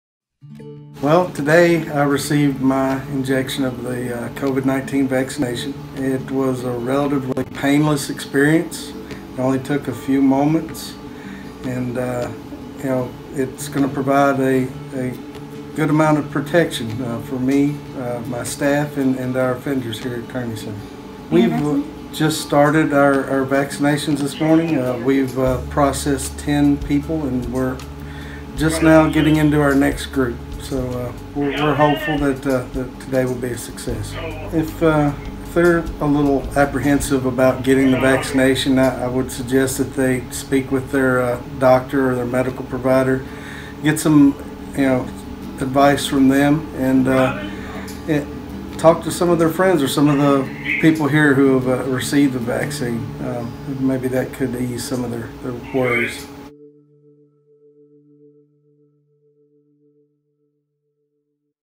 iGotMyShotTN | Warden Jason Clendenion
Jason Clendenion, newly appointed Warden for the Turney Center Industrial Complex in Only, TN tells us why he chose to get the COVID-19 vaccination.